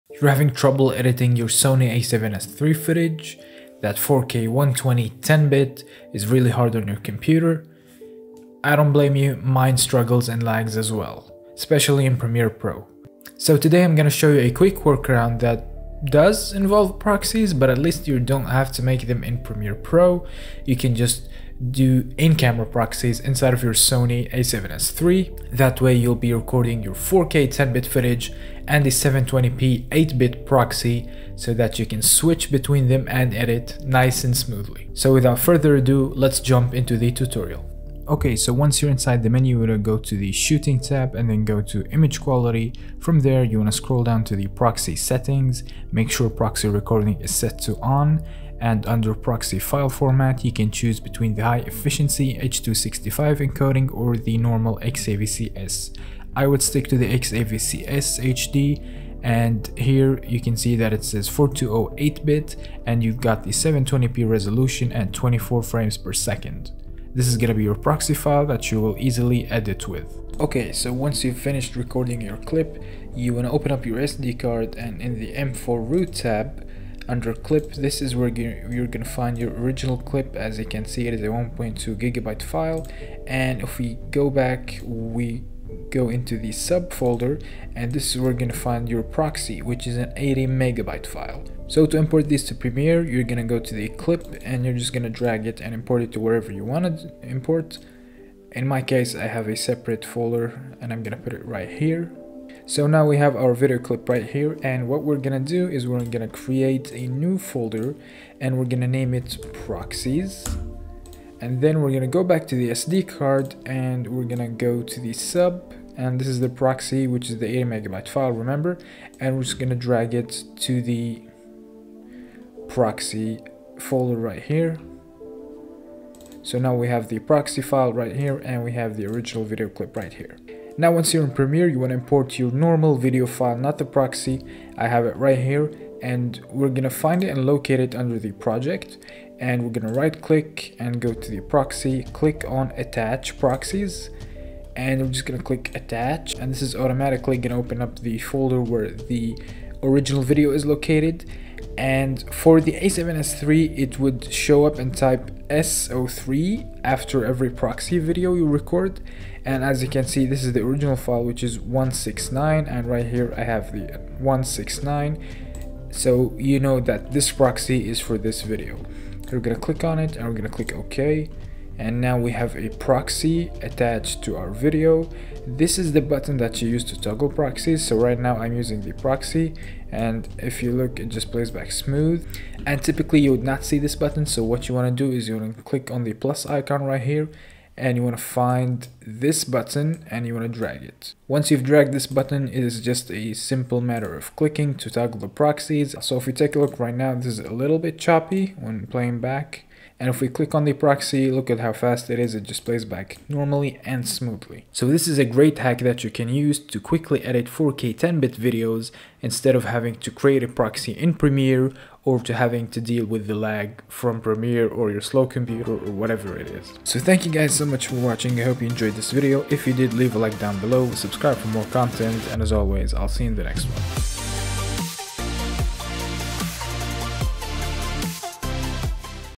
Record IN-CAMERA PROXIES | Sony A7SIII | Proxy Editing Workflow
Your computer can't edit 10-bit 4:2:2 footage? Here's a quick hack to easily edit Sony A7sIII footage on any computer.